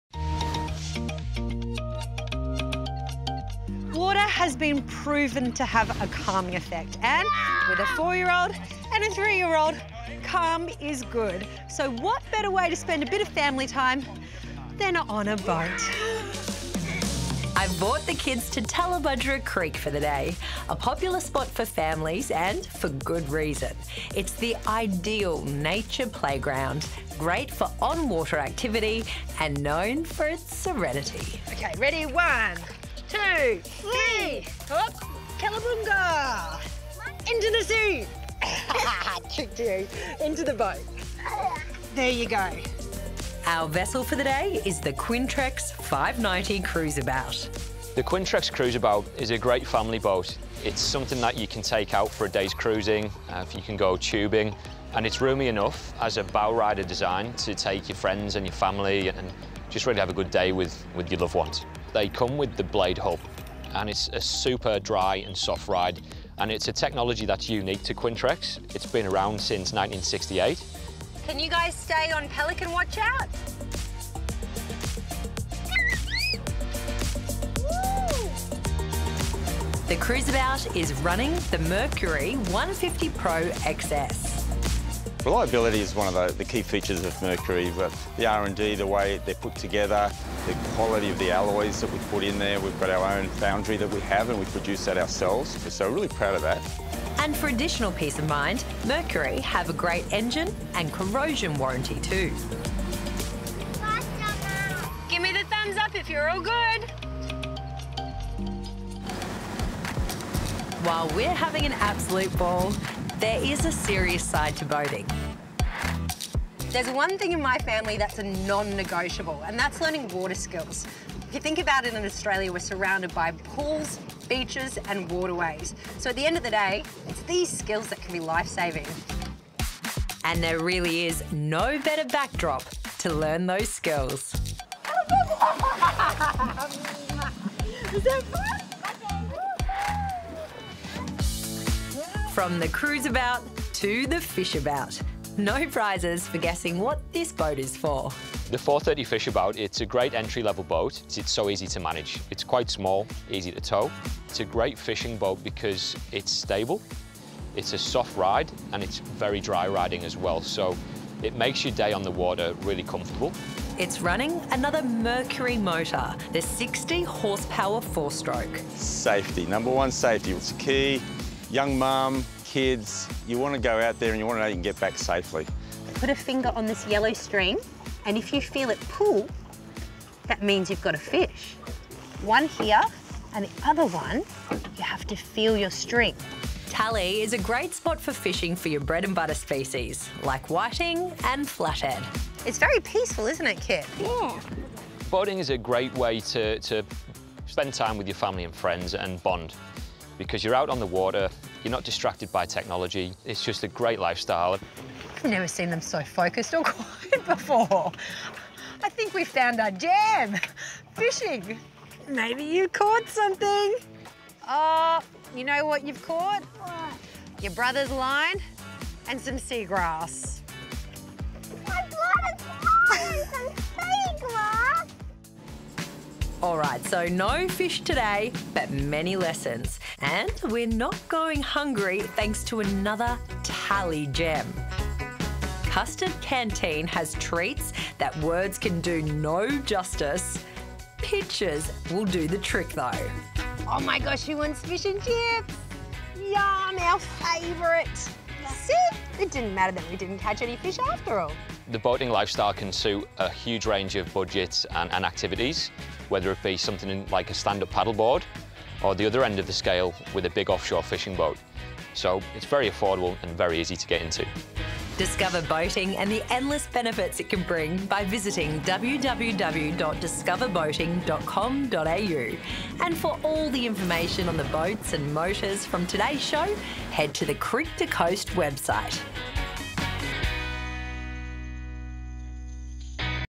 Tallebudgera Creek Boating with Creek to Coast, Qunitrex & Mercury Marine
There are so many benefits to the boating lifestyle. It’s a great way for families and loved ones to bond, without the distraction of technology. In Queensland, we’re lucky enough to have a variety of spots to choose from, like Tallebudgera Creek! ⚓🚤